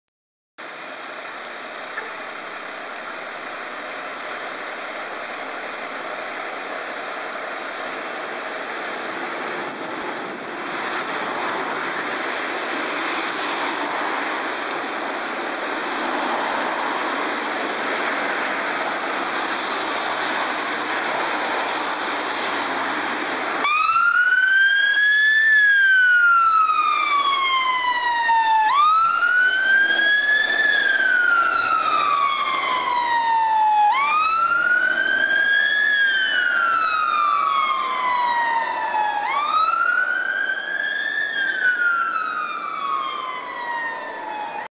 Eastbourne East Sussex Fire & Rescue Service
Eastbourne's ERP turning out to a fire call